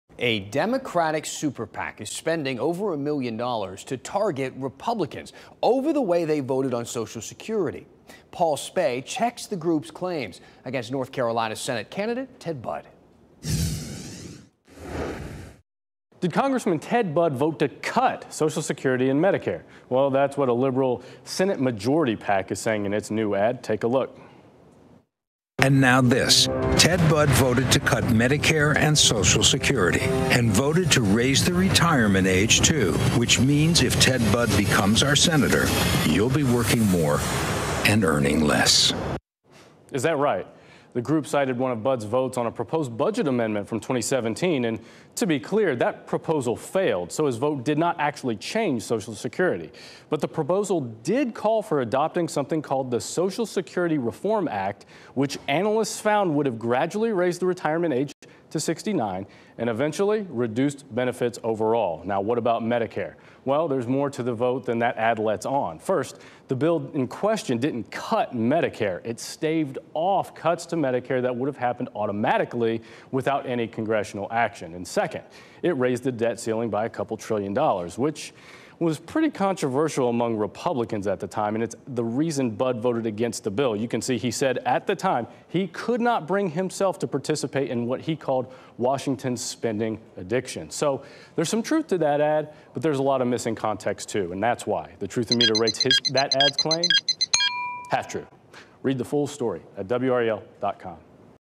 Fact Check: Did Budd vote to cut Social Security and Medicare?
A new ad by the Senate Majority PAC says U.S. Rep. Ted Budd "voted to cut Medicare and Social Security and voted to raise the retirement age too." PolitiFact checks the claim.

A top Democratic political group says a Republican U.S. Senate candidate could leave voters working more and earning less.

The Senate Majority PAC is targeting U.S. Rep. Ted Budd, who’s running against Democrat Cheri Beasley for the North Carolina seat of retiring Sen. Richard Burr. The ad, titled “Now this,” is one of several that the political action committee launched in late October attacking swing-state Republicans over their ideas for Social Security. In the ad, a narrator says:
“You already know Ted Budd’s family company screwed over North Carolina farmers. And Budd cosponsored a national abortion ban so out of the mainstream, it includes no exceptions for victims of rape or incest. And now this: Ted Budd voted to cut Medicare and Social Security and voted to raise the retirement age too—which means if Ted Budd becomes our senator, you will be working more and earning less.”